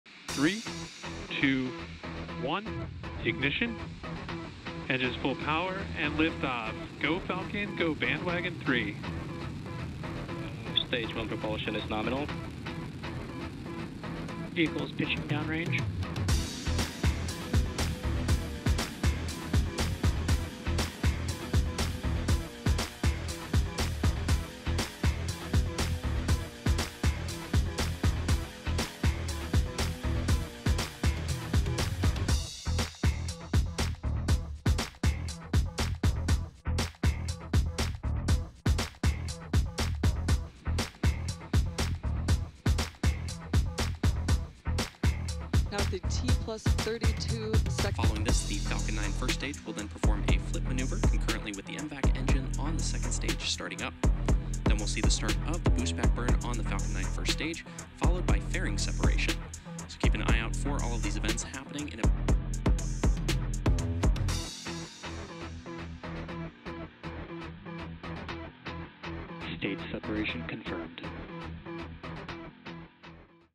‘북한 미사일 잡을 눈’ 군 정찰위성 4호기 지상교신 발사 성공
우리 군의 정찰위성 4호기가 22일 우주궤도 진입 후 지상국과의 교신에 성공했다.

정찰위성 4호기를 탑재한 미국 우주기업 스페이스Ⅹ의 발사체 '팰컨9'은 미국 플로리다주 케이프커내버럴 우주군 기지에서 한국시간으로 이날 오전 9시 48분(현지시간 21일 오후 8시 48분)에 발사됐다.